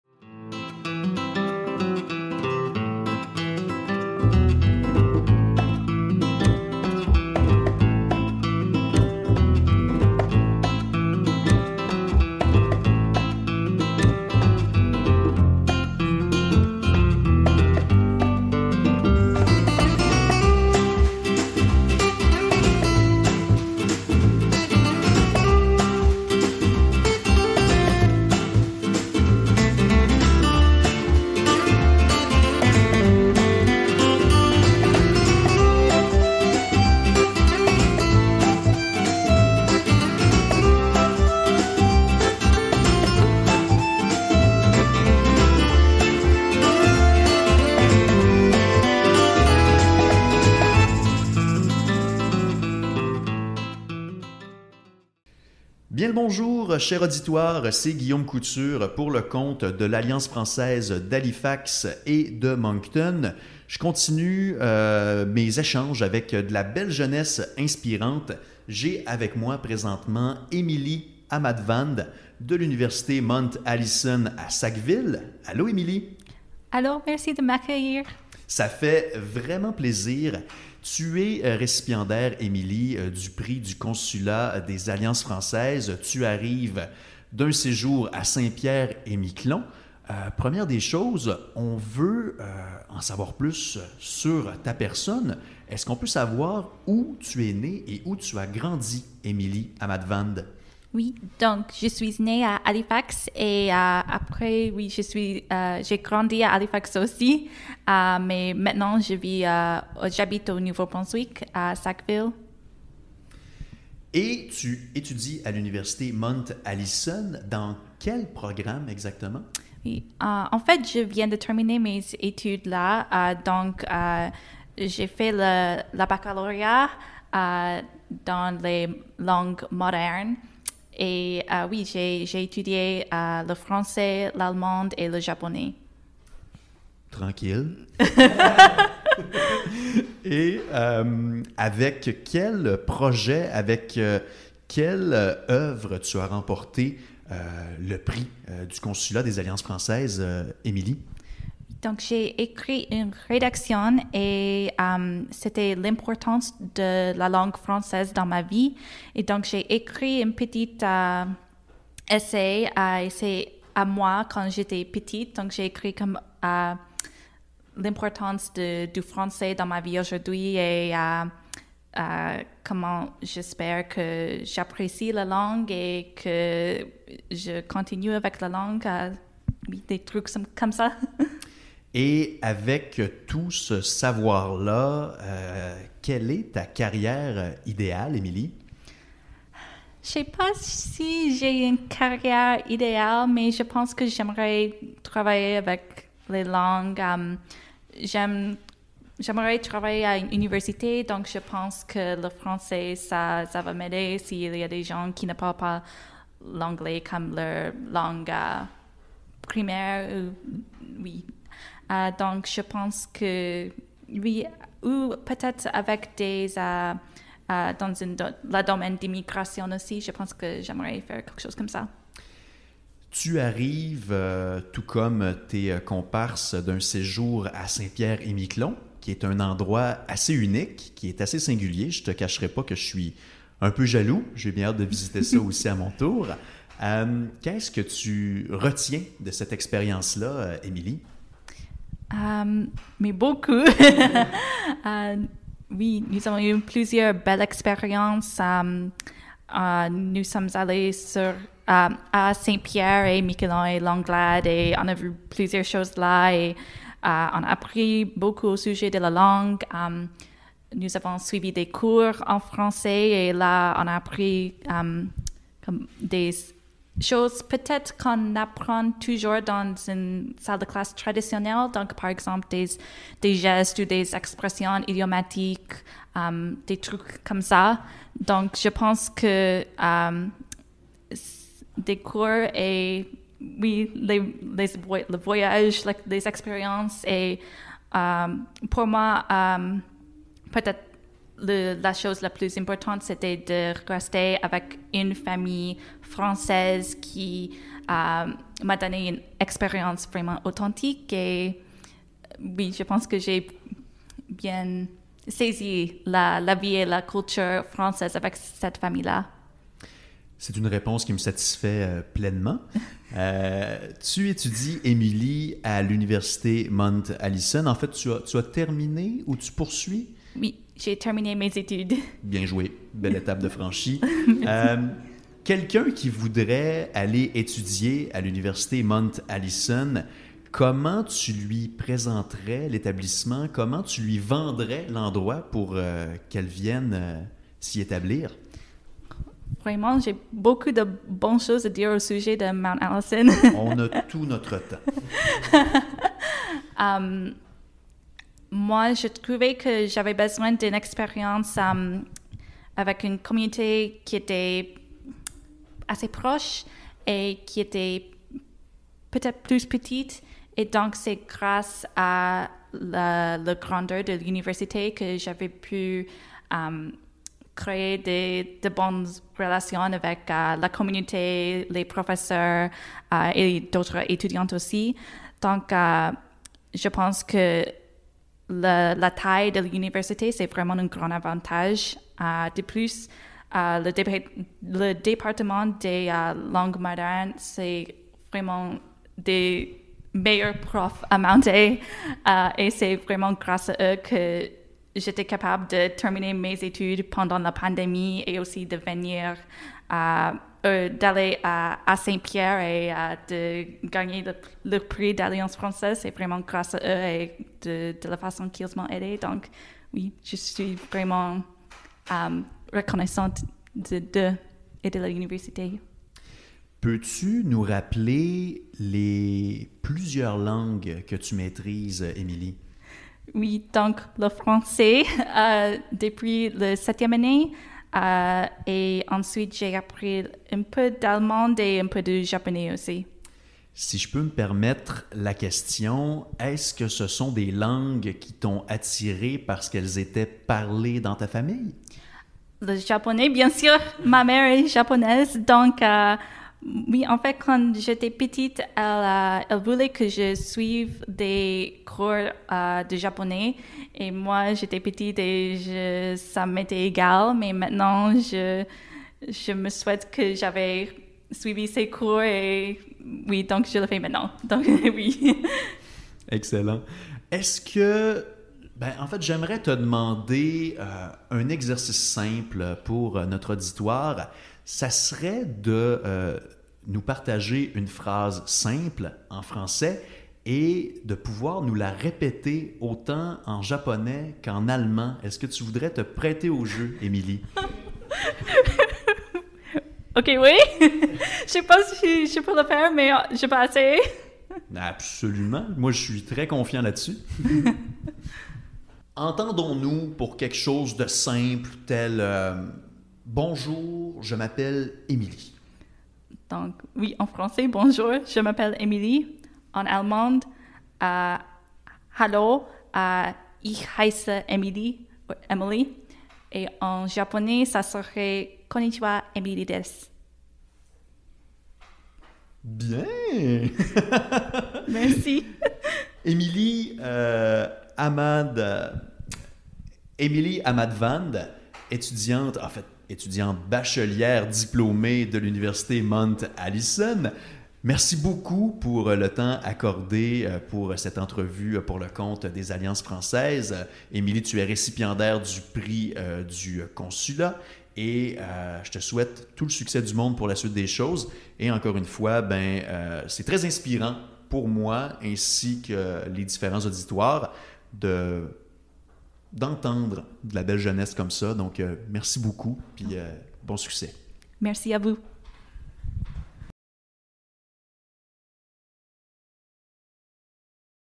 Emily Ahmadvand - Prix du Consulat général de France dans les Provinces atlantiques 2022
Le Prix du Consulat général de France dans les Provinces atlantiques découle du Prix des Alliances Françaises de Moncton et Halifax. Ce Prix d'excellence a pour objectif de récompenser les étudiant.e.s universitaires des programmes de langue française des universités des 4 Provinces atlantiques.

Les lauréat.e.s du Prix du consulat gagnent un séjour linguistique et culturel d'une semaine au Francoforum de Saint-Pierre-et-Miquelon. Voici le retour d'expérience de Emily Ahmadvand, lauréate 2022 et étudiante de Mount Allison University au Nouveau-Brunswick.

Entretien réalisé par Guillaume Couture, journaliste/animateur à la radio communautaire "Oui 98.5 FM".